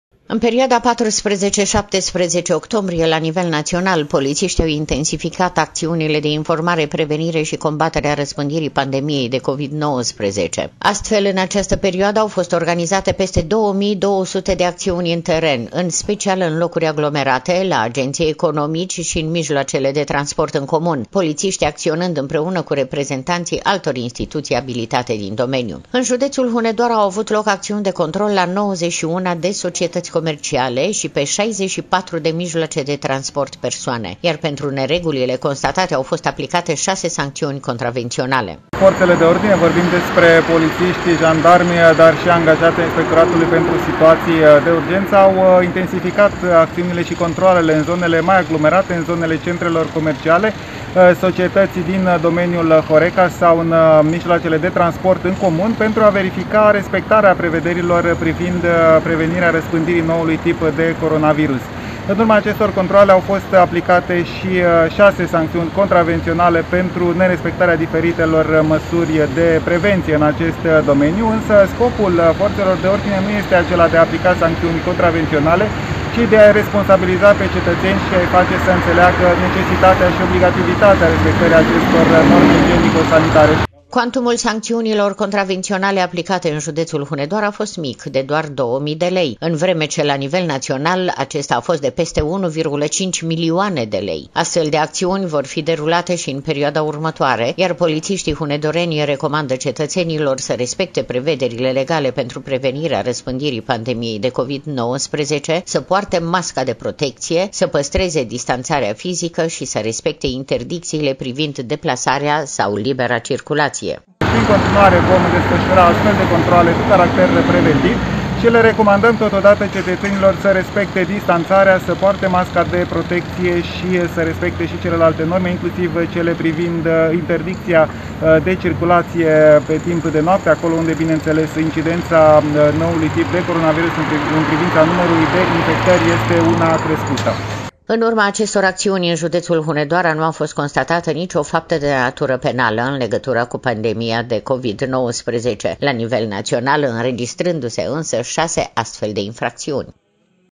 POLIȚIȘTII HUNEDORENI, IMPLICAȚI ÎNTR-O ACȚIUNE NAȚIONALĂ ÎMPOTRIVA COVID
În perioada 14 – 17 octombrie, la nivel național, polițiștii au intensificat acțiunile de informare, prevenire și combatere a răspândirii pandemiei de COVID-19. Astfel, în această perioadă, au fost organizate peste 2.200 de acțiuni în teren, în special în locuri aglomerate, la agenți economici și în mijloacele de transport în comun, polițiștii acționând împreună cu reprezentanții altor instituții abilitate din domeniu. În județul Hunedoara au avut loc acţiuni de control la 91 de societăţi comerciale și pe 64 de mijloace de transport persoane, iar pentru neregulile constatate au fost aplicate șase sancţiuni contravenţionale.